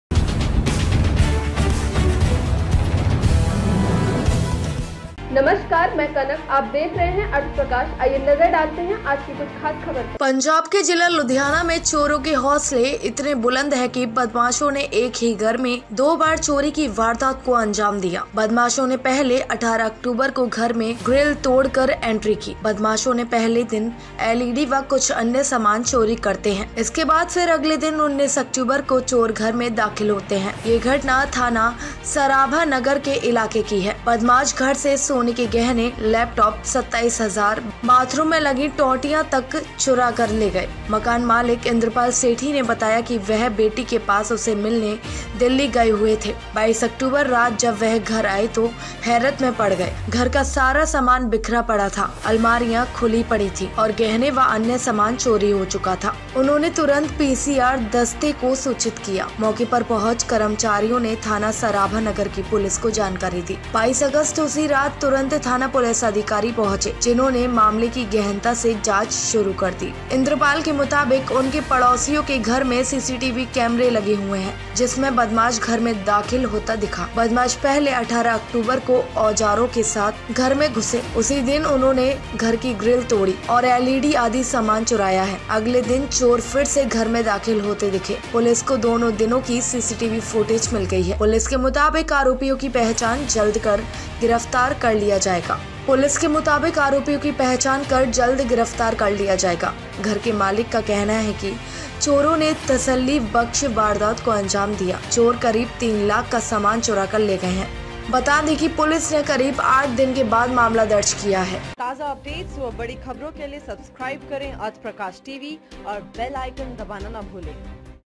चोरों ने हौंसले इतने बुलंद है कि बदमाशों ने एक ही घर में दो बार चोरी की वारदात को अंजाम दिया
ये घटना थाना सराभा नगर के इलाका की है। बदमाश घर से सोने के गहने, लैपटाप, 27 हजार, बाथरूम में लगी टूटियां तक चुरा कर ले गए। मकान मालिक इंद्रपाल सेठी ने बताया कि वह बेटी के पास उसे मिलने दिल्ली गए हुए थे। 22 अक्तूबर रात जब वह घर आए तो हैरत में पड़ गए। घर का सारा सामान बिखरा पड़ा था। अलमारियां खुली पड़ी थी और गहने व अन्य सामान चोरी हो चुका था।उन्होंने तुरंत PCR दस्ते को सूचित किया। मौके पर पहुंच कर्मचारियों ने थाना सराभा नगर की पुलिस को जानकारी दी। 22 अगस्त उसी रात तुरंत थाना पुलिस अधिकारी पहुंचे जिन्होंने मामले की गहनता से जांच शुरू कर दी। इंद्रपाल मुताबिक उनके पड़ोसियों के सीसीटीवी कैमरा लगा है जिसमें बदमाश घर में दाखिल होता दिखा। बदमाश पहले 18 अक्तूबर को औजारों के साथ घर में घुसे।

उसी दिन उन्होंने घर की ग्रिल तोड़ी और LED आदि सामान चुराया है। अगली दिन चोर फिर से घर में दाखिल होते दिखे। पुलिस को दोनों दिनों की सीसीटीवी फूटेज मिल गई है। पुलिस मुताबिक आरोपियों की पहचान कर जल्द गिरफ्तार कर लिया जाएगा। घर के मालिक का कहना है कि चोरों ने तस्सली बख्श वारदात को अंजाम दिया। चोर करीब तीन लाख का सामान चुराकर ले गए है। बता दें पुलिस ने करीब 8 दिन बाद मामला दर्ज किय है